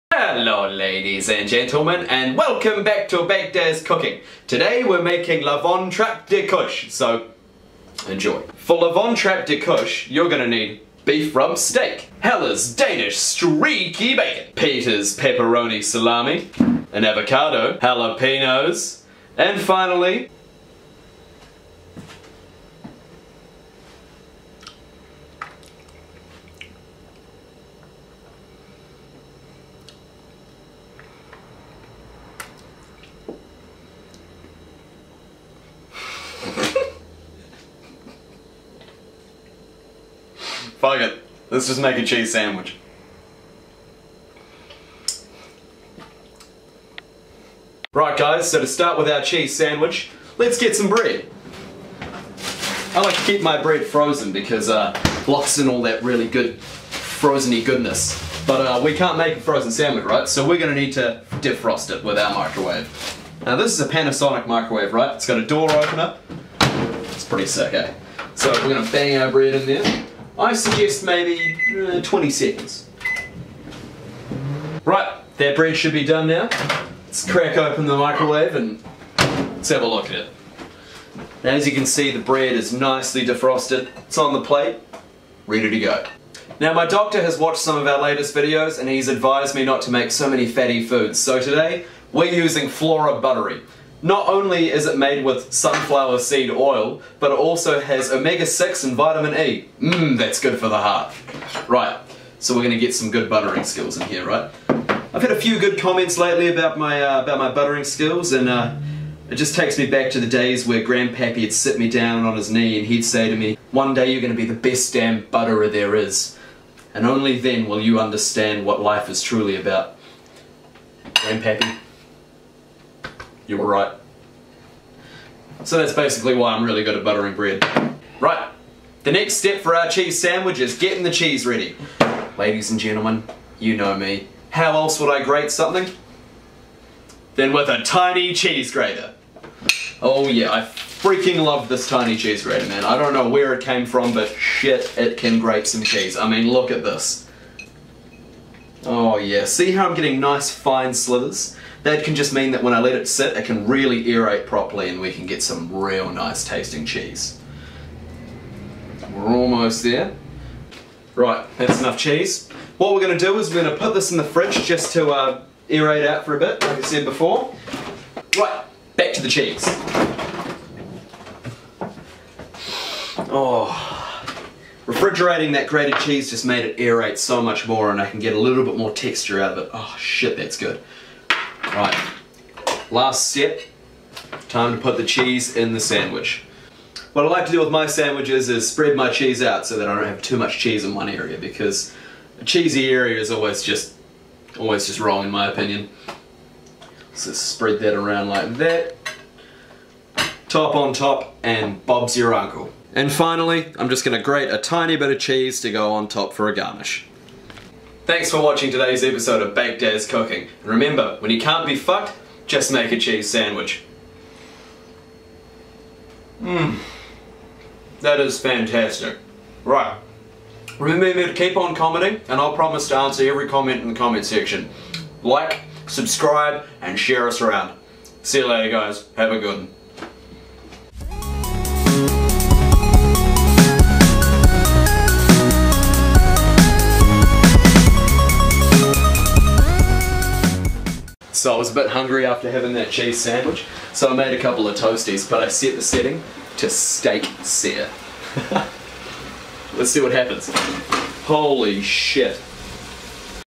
Baked As Cooking: "La Von Trapp de Kush"
Episode three of Baked as Cooking and our chef is whipping us up a new "Fantastic" meal for us, "La Von Trapp de Kush... or something like that.

Got a recipe you want to see the chef attempt?